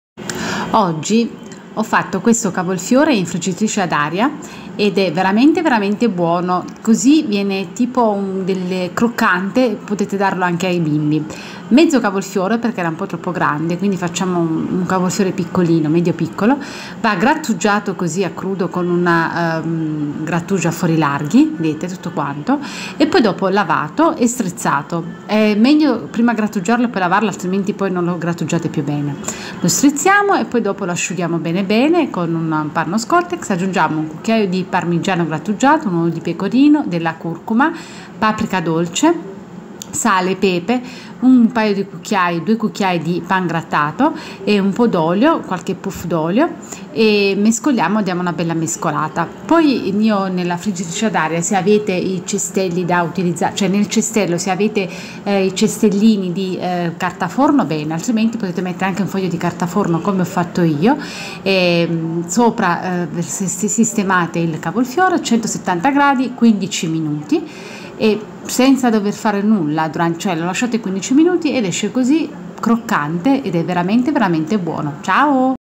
Cavolfiore gratinato in friggitrice ad aria
mezzo Cavolfiore grande o uno medio piccolo
1 cucchiaio di parmigiano grattugiato
1 cucchiaio di pecorino grattugiato
2 cucchiai di pangrattato
sale e pepe q.b
curcuma e paprika dolce q.b
3/4 puff di olio evo

Grattugiare il cavolo con grattugia a fori larghi; lavare e strizzare il cavolfiore ; aggiungere gli ingredienti e dare una mescolata!Foderare il cestello della friggitrice con carta forno e cuocere a 170° per 15 minuti.

È buonissimo!!